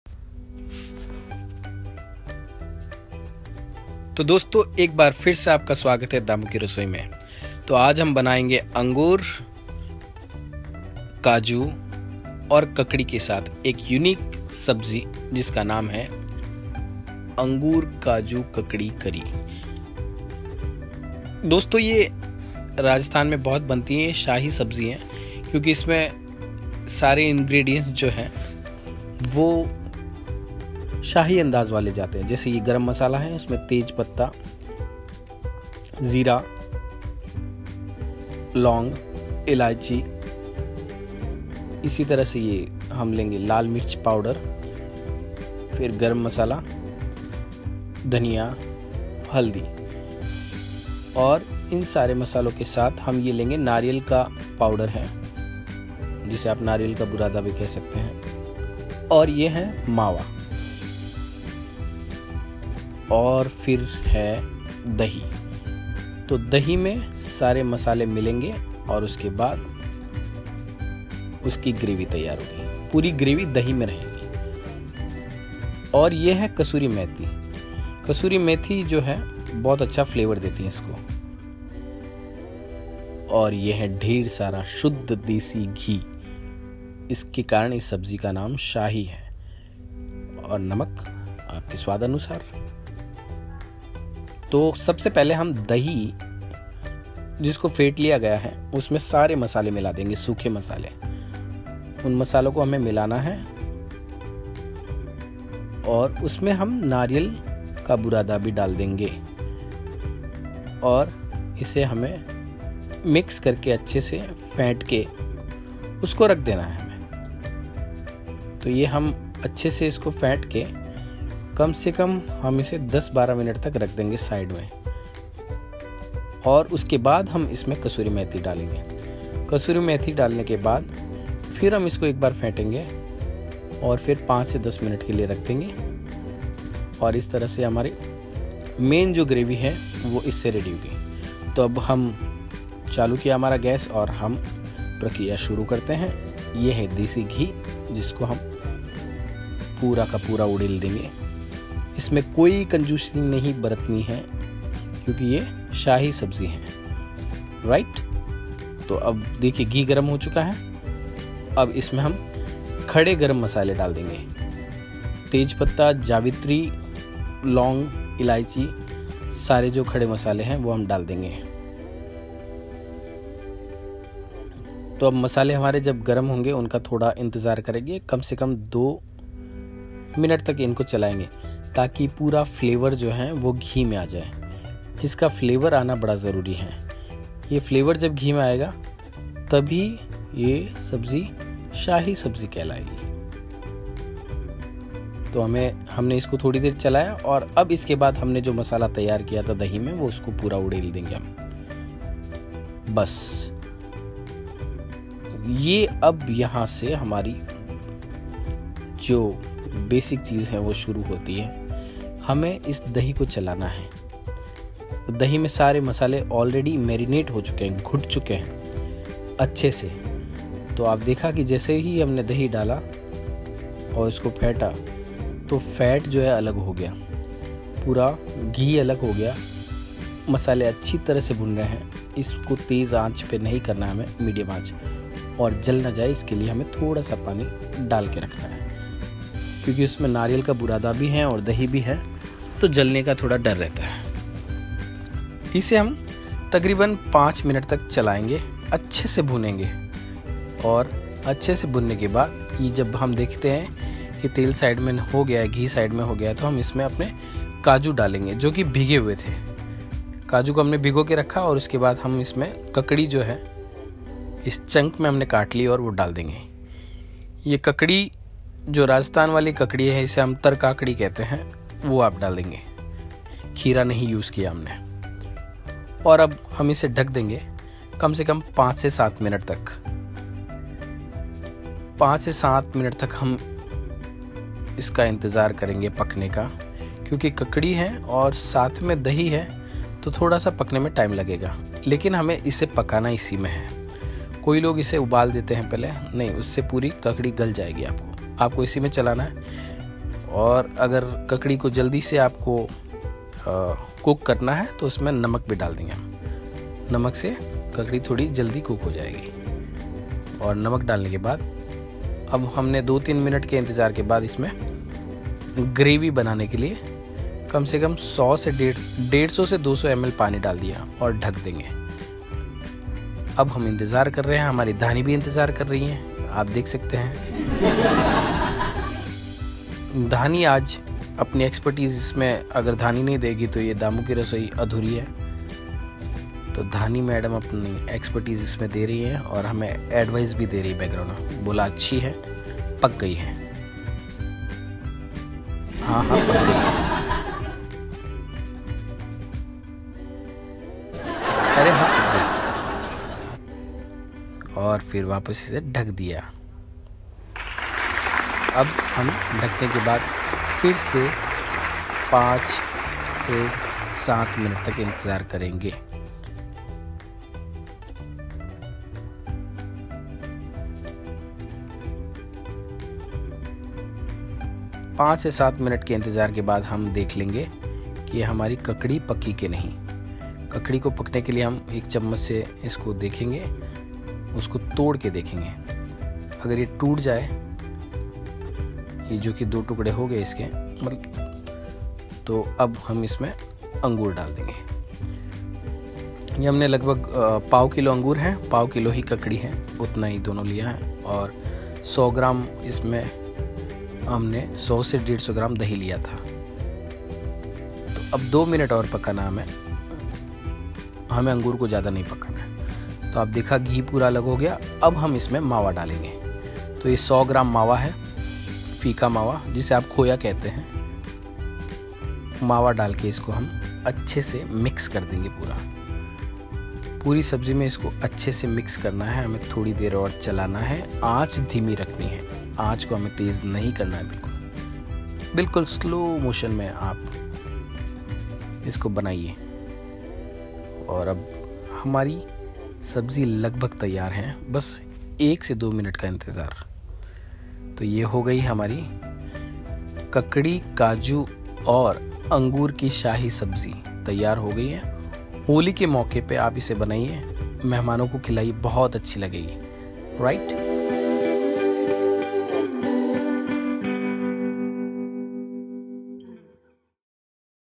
ये सब्ज़ी आपने कभी नहीं खाई होगी ll शाही काजू ककड़ी अंगूर ll
एक शानदार शाही सब्ज़ी जो बिल्कुल देसी घी में बनती है। इसमें मावा और मेवे पड़ते हैं। राजस्थान में ख़ास मौक़ों पर ये सब्ज़ी बनती है। हमने एकदम देसी अन्दाज़ में बनाई है। गैरंटी है आप उँगलिया चाटते रह जाएँगे।

रेसिपी में इस्तेमाल सभी सामग्री का माप-तोल विडीओ में ही दिया गया है।

आप हमें Facebook, Instagram और Twitter पर भी DamuKiRasoi के नाम से सर्च कर सकते हैं।